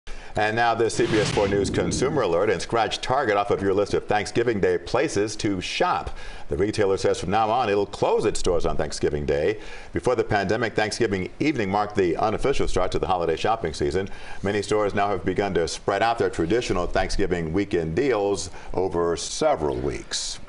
Target Will Never Open On Thanksgiving Again
To limit crowds in stores, retailers last year were forced to turn what had become a weekend shopping blitz into an extended event, with big holiday discounts beginning as early as October. That forced shift appears to have been fortuitous.